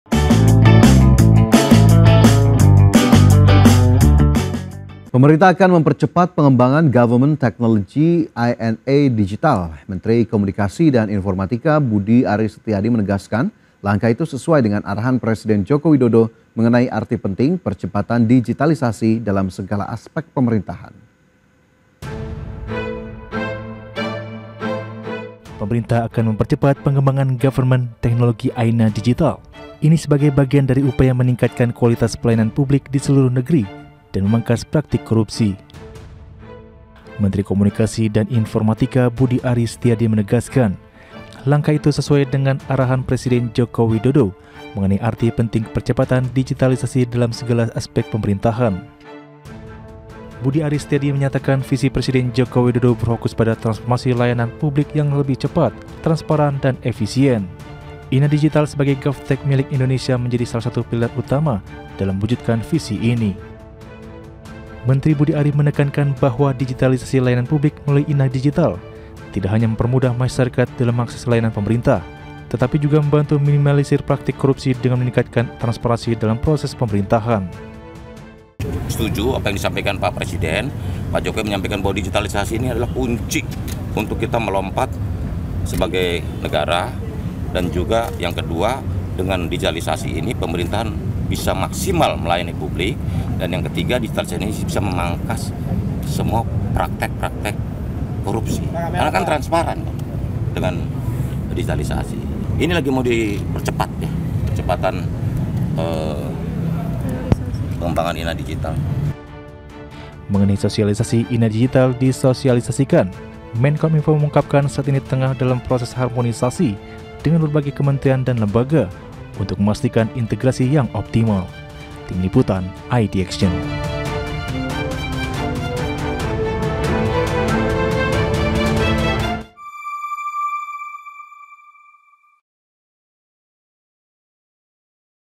Menkominfo: Pengembangan INA Digital Dipercepat | IDX CHANNEL
Pemerintah akan mempercepat, pengembangan Government Technology INA Digital. Menteri Kominfo, Budi Ari Setiadi menegaskan, langkah itu sesuai dengan arahan Presiden Joko Widodo mengenai arti penting percepatan digitalisasi dalam segala aspek pemerintahan.